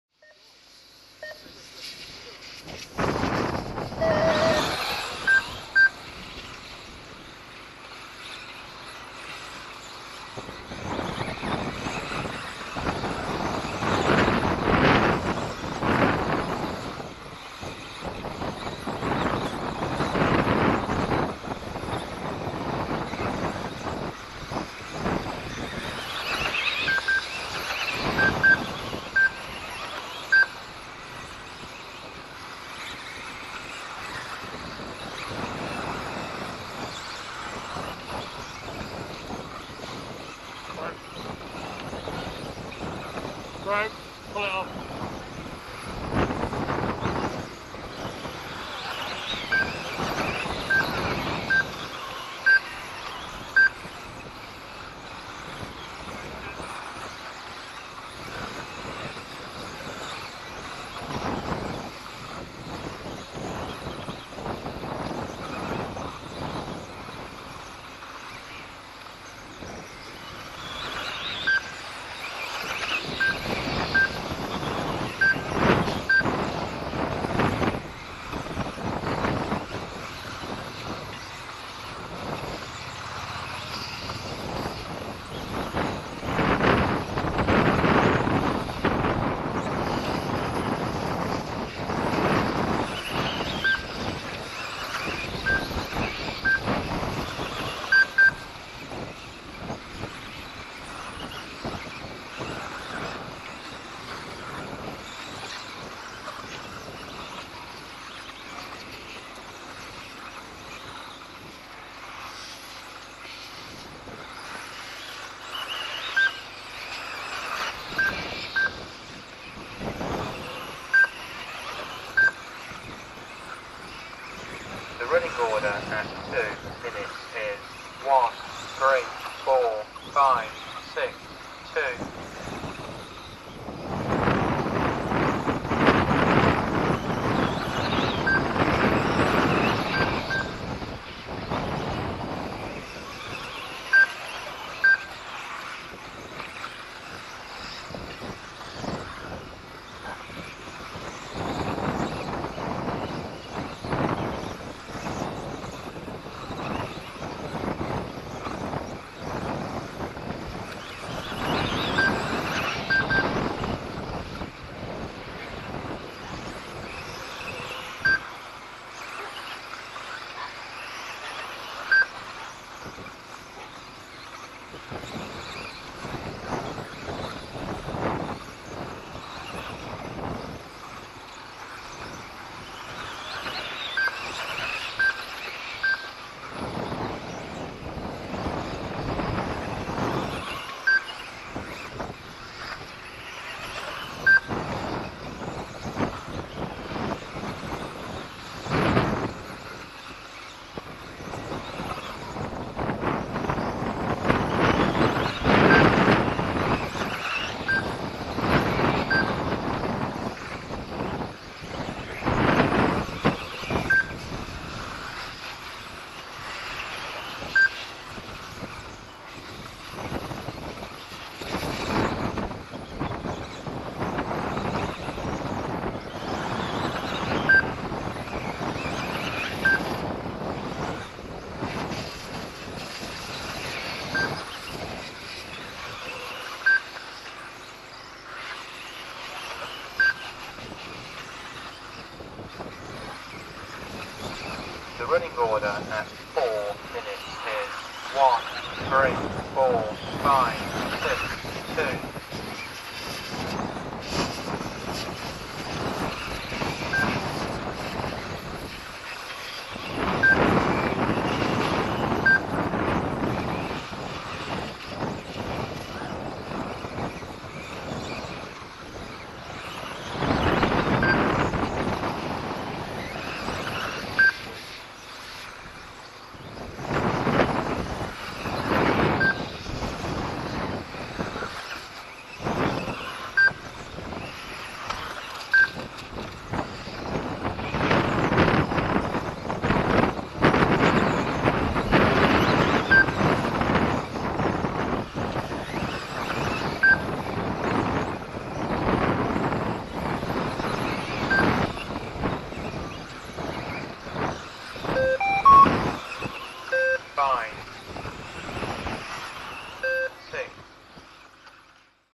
Mendip - 2WD A Final Leg 3 - 04/04/21
2WD A Final Leg 3 @ Mendip. Started 5th and finished 4th